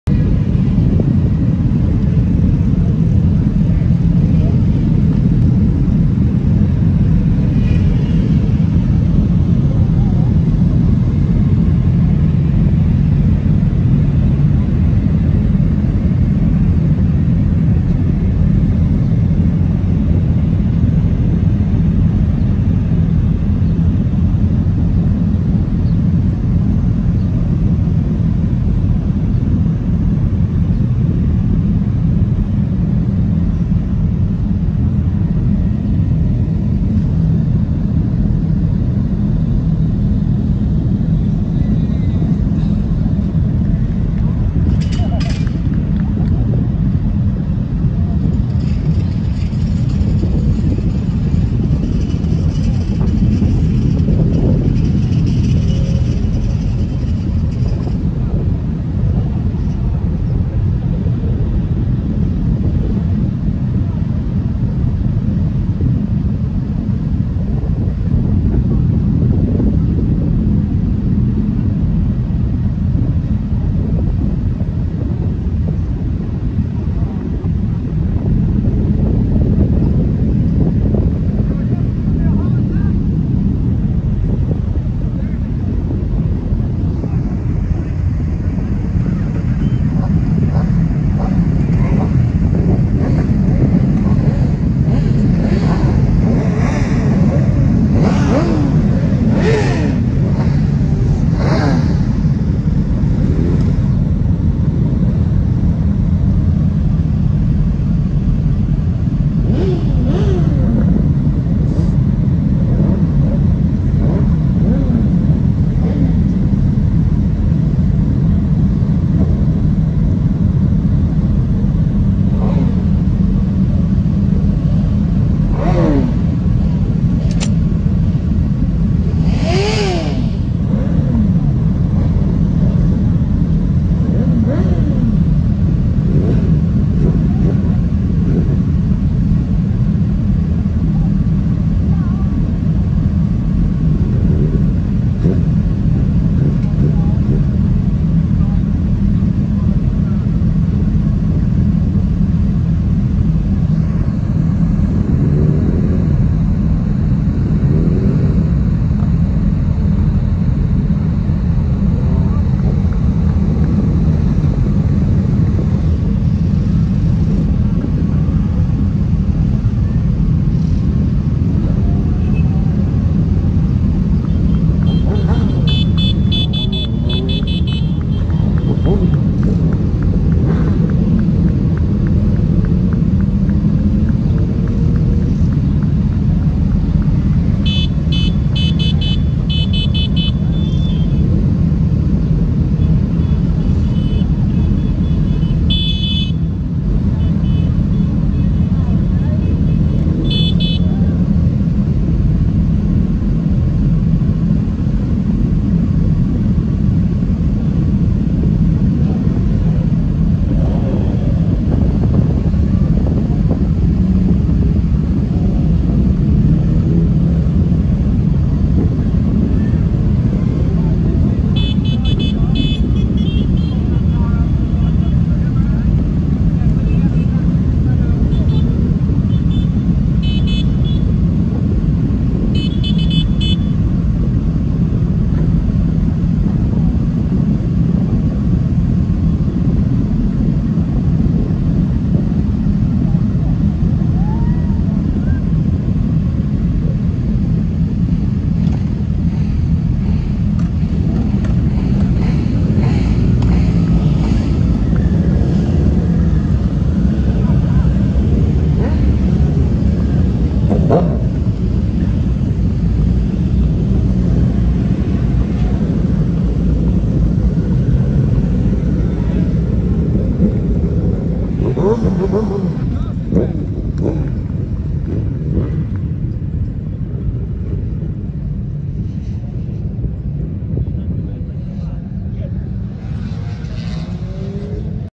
Honda fireblade lap start Saturday | BSB Donington May 2022
Bikes participating in Honda’s celebration of 30 years of the Fireblade at Donington Park Round 3 of BSB 2022